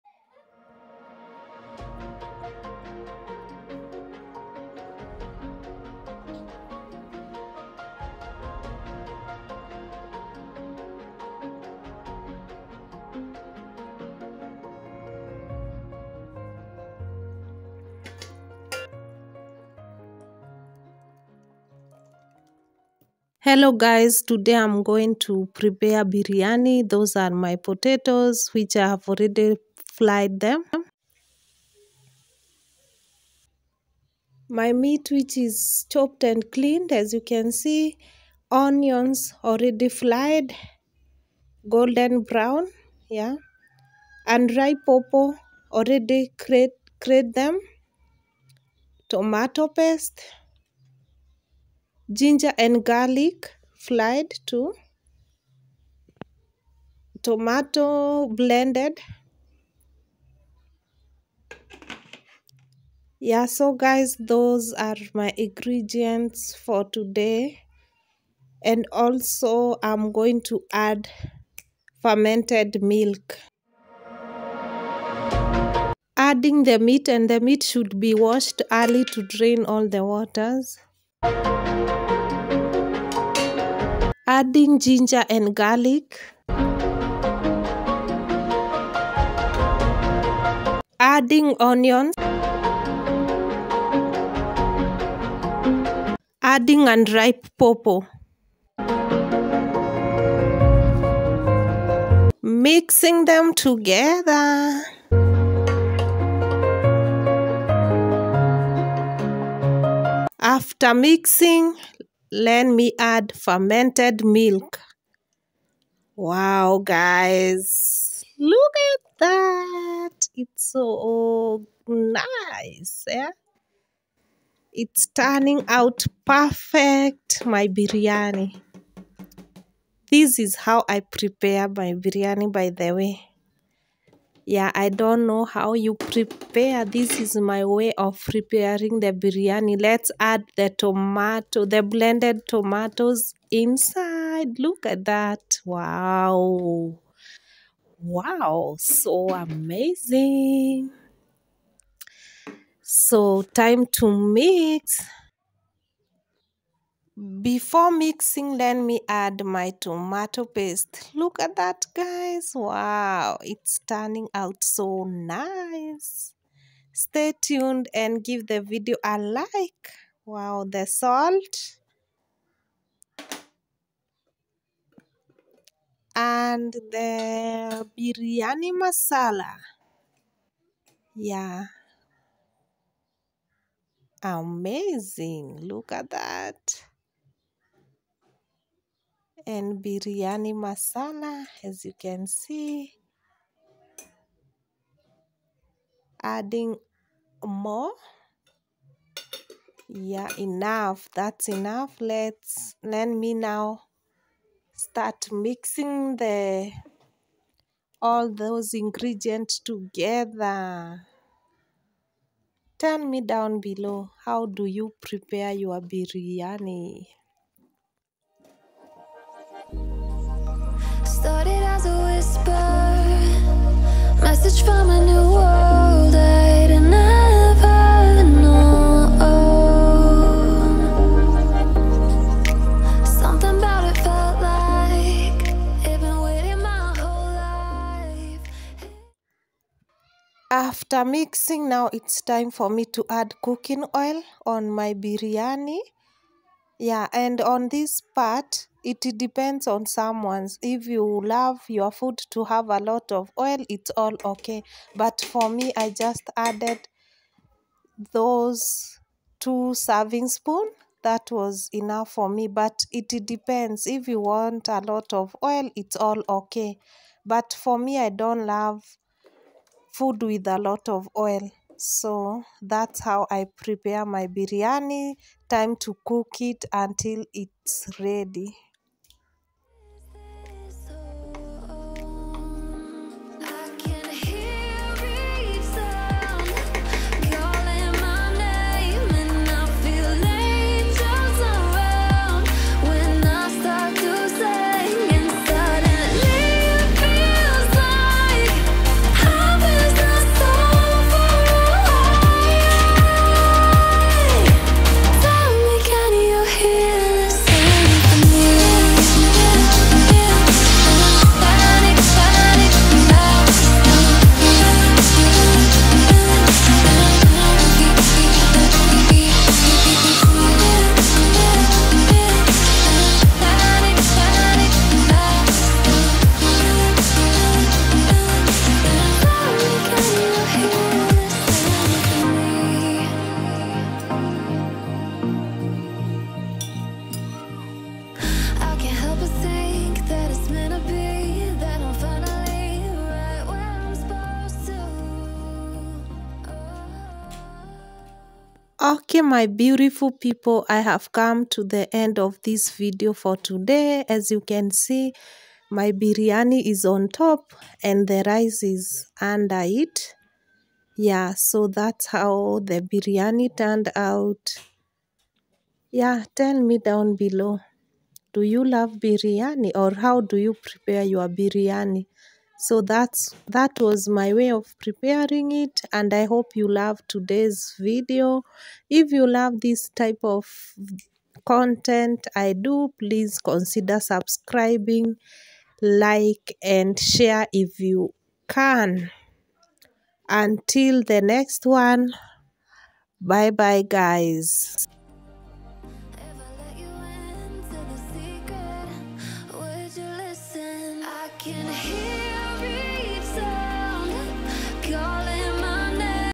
Simple Biriyani Recipe🤤🤤. biriyani cookingvlog.
Biriyani is a Swahili meal which is easy to prepare